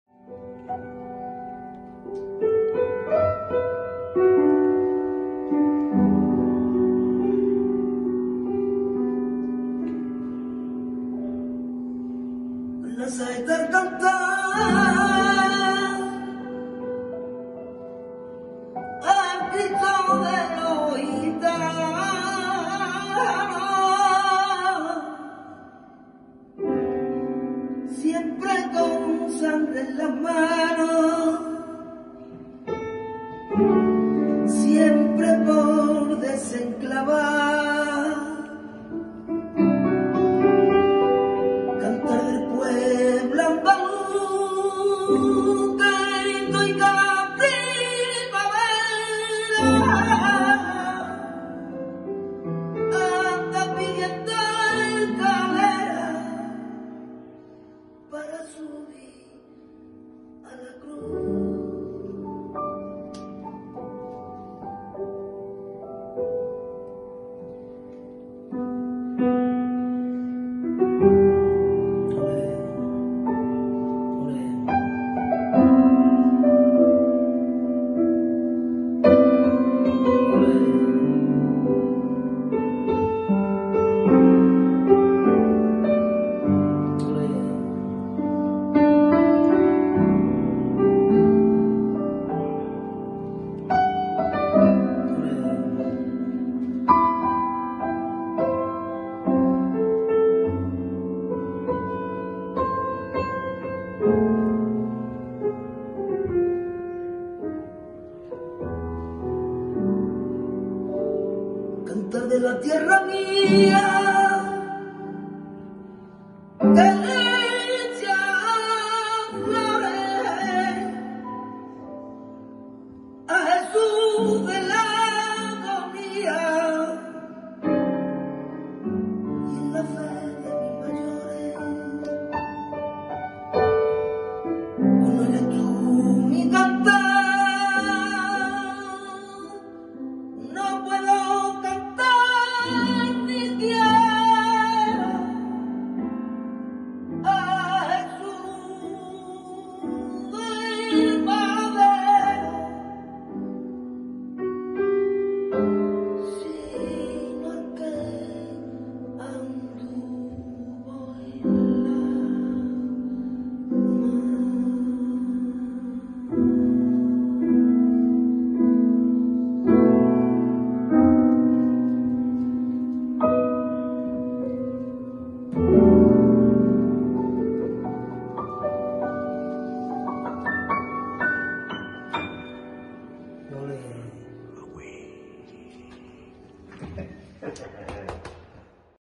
Feliz Lunes de Pascua a todos 🙏🏽➕🙌🏽🤍Saeta Rafael de Utrera
Saeta en la voz de Rafael de Utrera y piano.